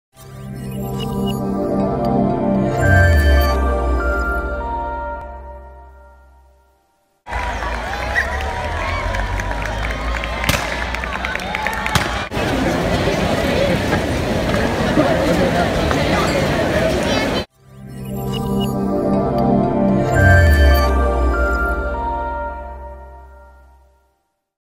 III Ruta Solidaria del Rey Mago a Covadonga
Más de 600 personas participaron en la tercera edición de la Ruta Solidaria del Rey Mago, entre Cangas de Onís y Covadonga, que recauda juguetes para entregar a varias ONG. Cada participante lleva consigo un juguete.